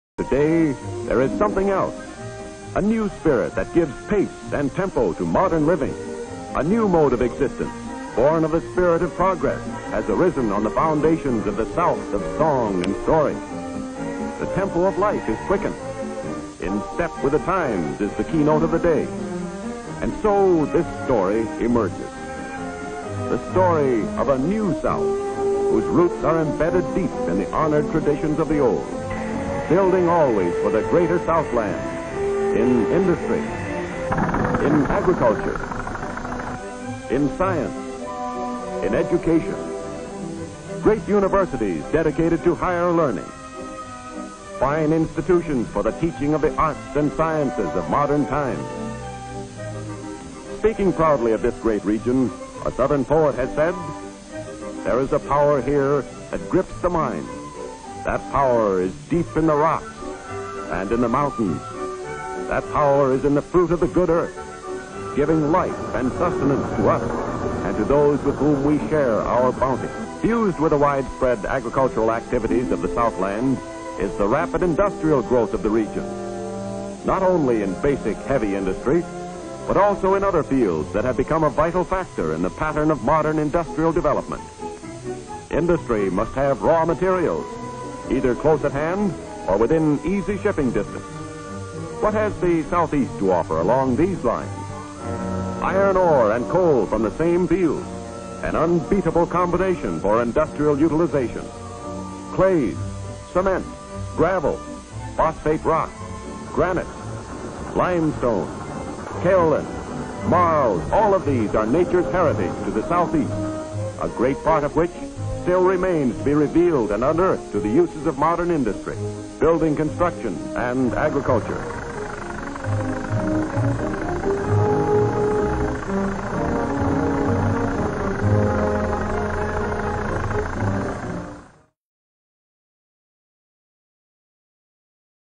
Iron ore steel mills Ensley, Birmingham, Alabama
Vintage film 1948 Ensley, Birmingham, Alabama showing steel mills and industrial development.